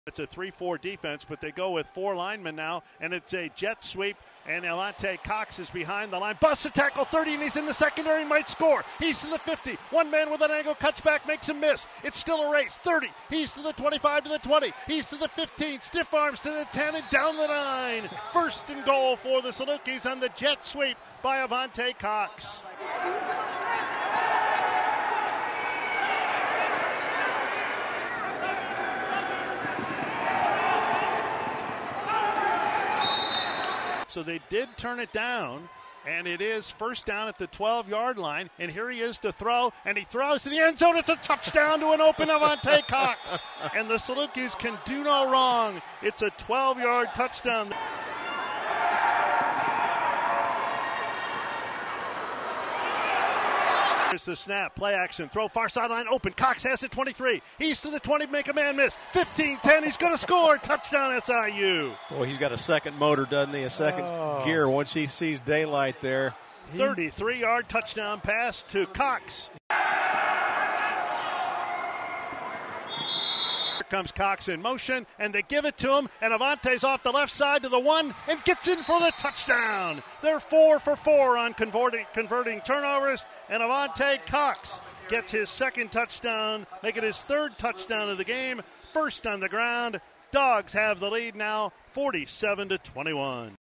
Avante Cox — MVFC Player of the Week
Salukis WR Avante Cox won the MVFC Player of the Week and Newcomer of the Week award for his performance at South Dakota.